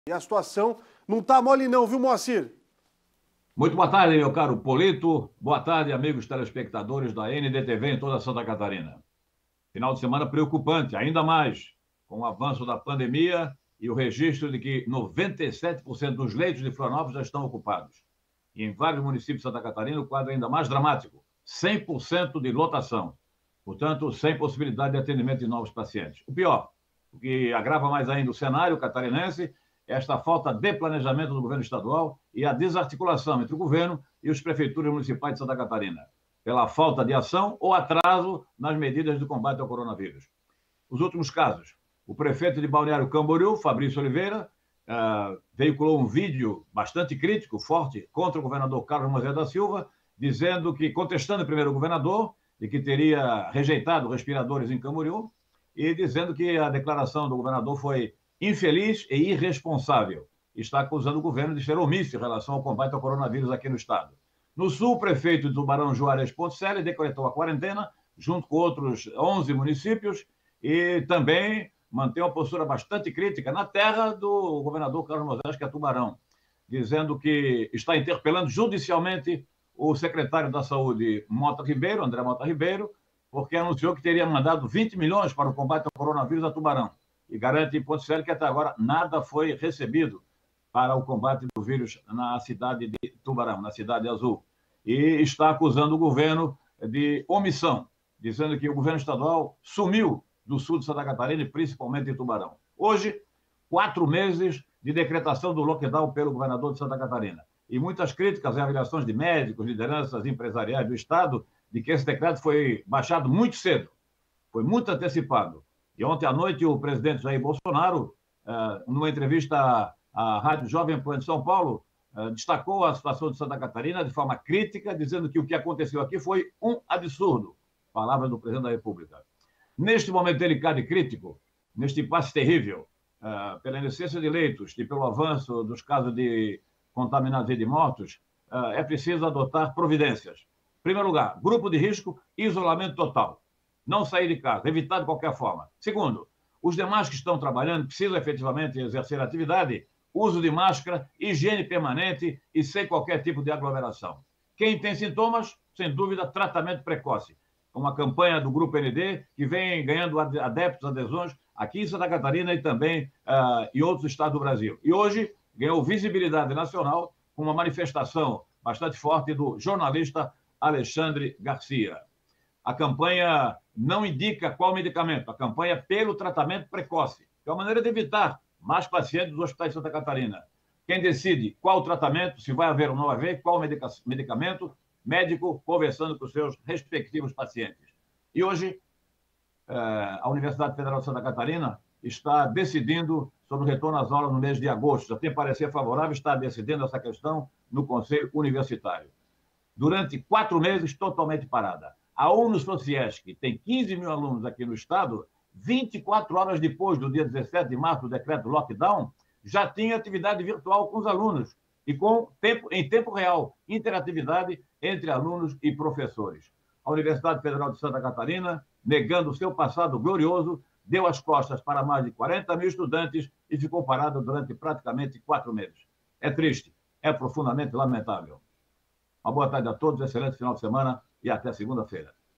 "A UFSC deu as costas para 40 mil estudantes", diz Moacir Pereira sobre retorno das aulas
Confira o comentário desta sexta-feira (17) de Moacir Pereira sobre o retorno das aulas na pandemia.